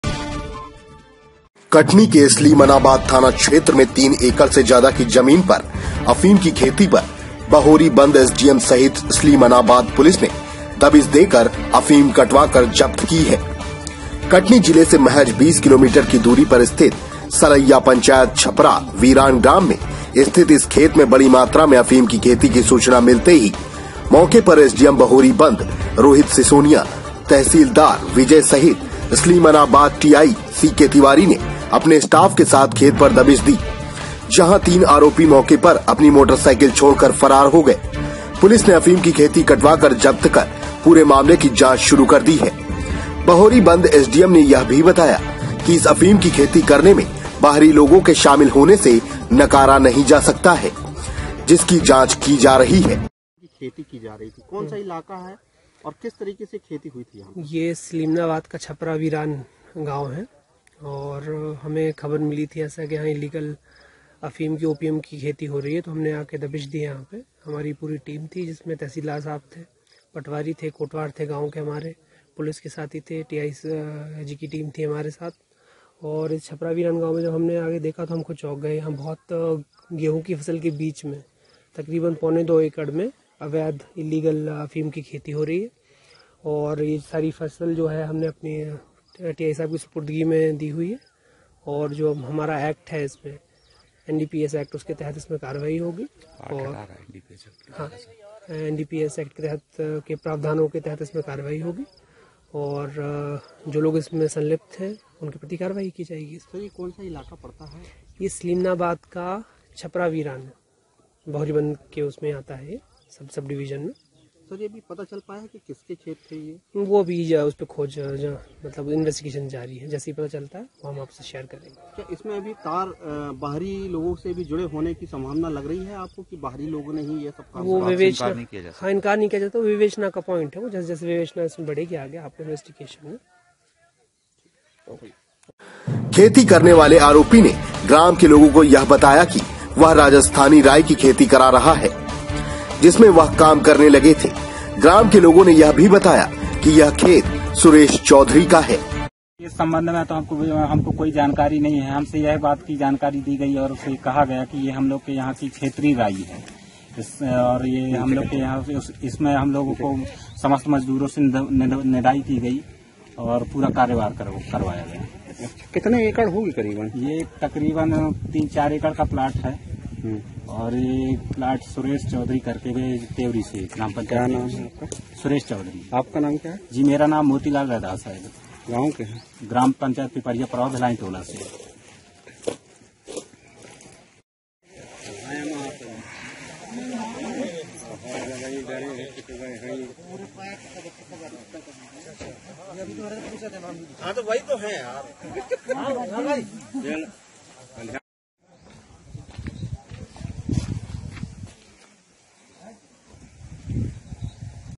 3 एकड़ में हो रही अफीम की खेती पर पुलिस का छापा... पुलिस ब्यूरोक्रेट्स न्यूज 25 फरवरी
Kunal Singh Editor in Chief